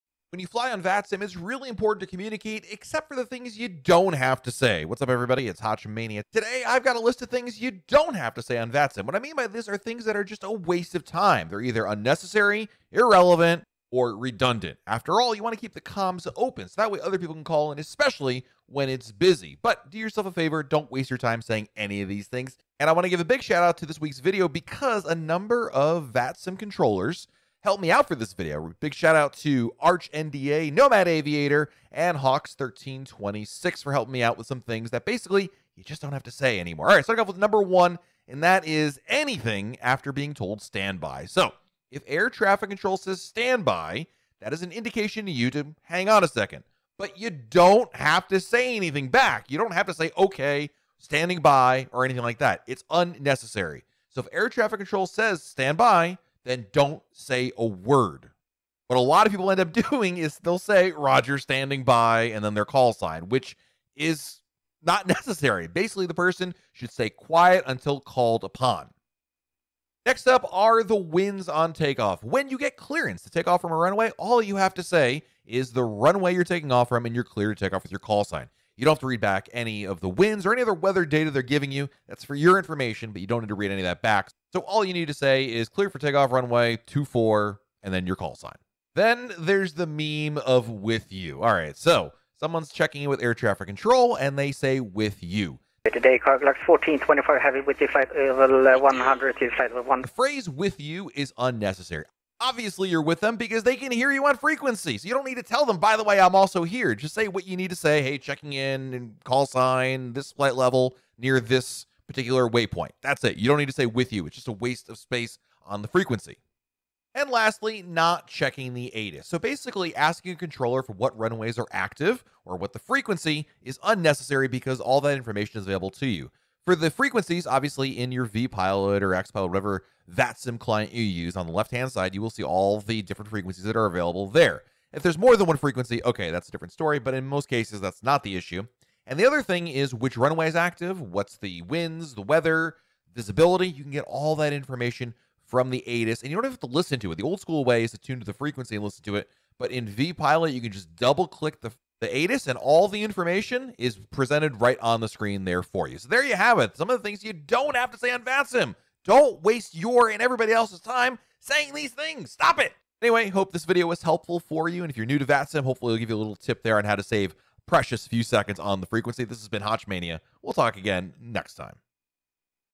Things Pilots Say On VATSIM That Are UNNECESSARY!
Flying on VATSIM means communicating well with pilots and air traffic control, but there are a things plenty of pilots say on Vatsim that they dont need to say at all!

Thank you to these VATSIM ATC controllers for their help!